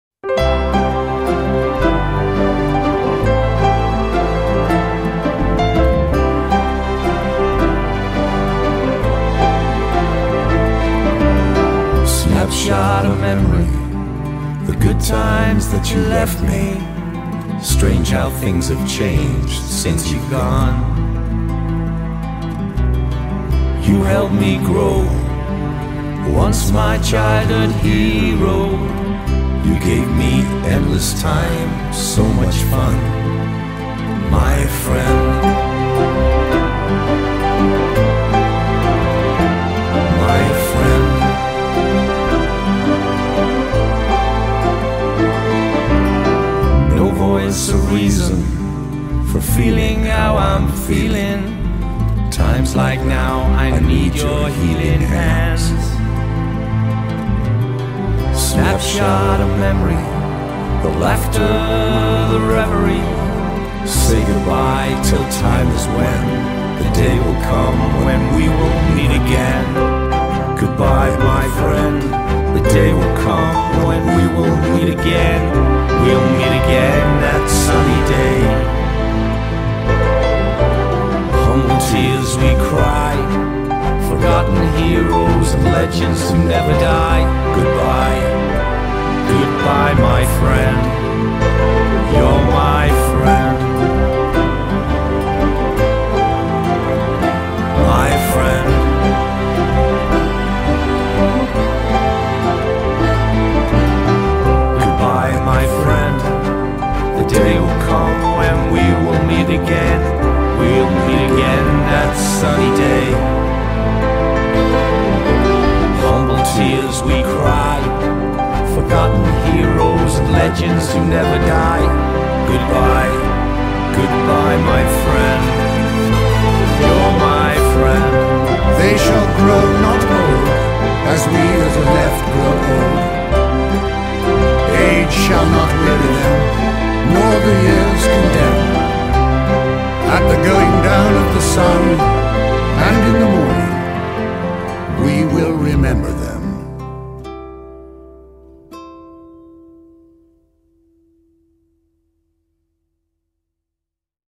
Reverie
Reverie, a state of being lost in your own thoughts. A daydream.
76 years ago today thousands of brave souls landed on the beaches of Normandy and gave their lives. The Time Bandits would like to pay tribute to them with this show reel.